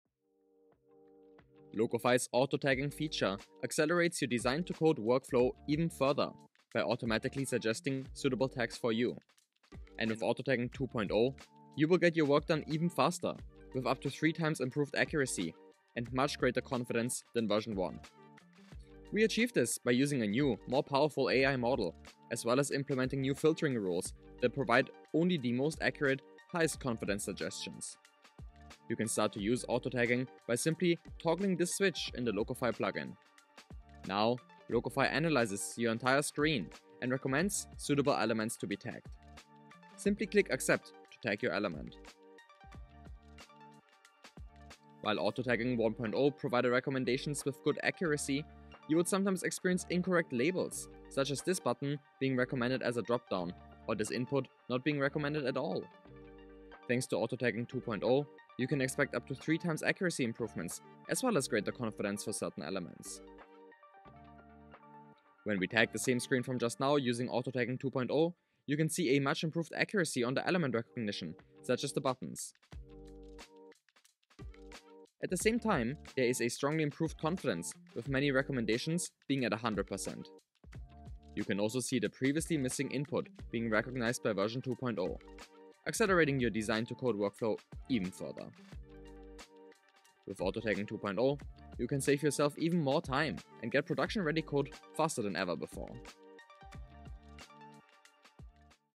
Locofy v6.4 - Auto-Tagging 2.0
📣 Introducing Auto-tagging 2.0 for up to 3x improved accuracy & confidence!

✅ Experience enhanced tag recognition and boosted confidence levels with many elements now at 100%.
✅ Leveraging a powerful new AI model and implementing refined filtering rules, we have significantly improved accuracy and confidence levels.

Build 5-10x faster with Locofy.ai⚡️